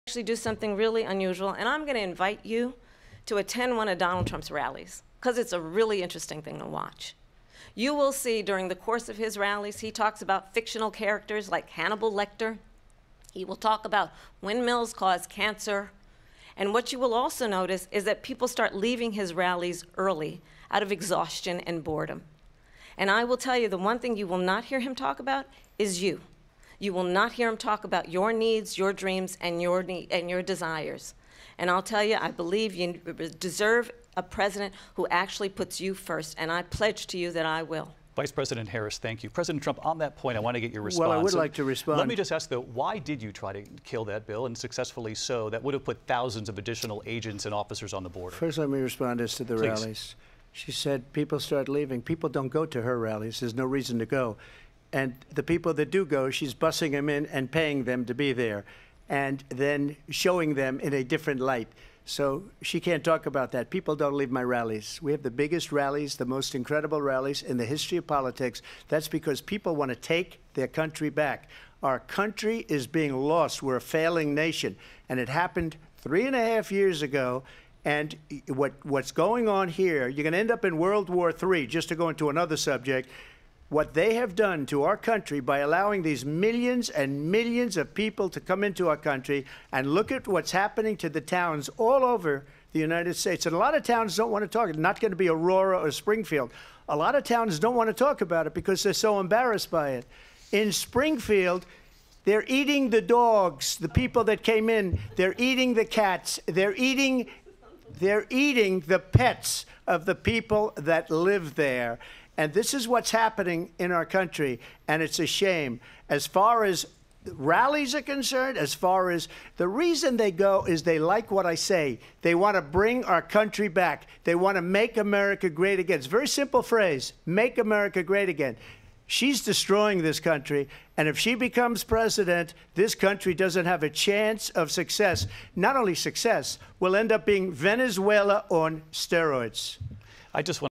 "People leave his rallies early out of exhaustion and boredom." - KAMALA HARRIS to TRUMP in debate
VP Harris: "People start leaving his rallies early out of exhaustion and boredom."

Former President Trump: "People don't go to her rallies...people don't leave my rallies. We have the biggest rallies, the most incredible rallies in the history of politics."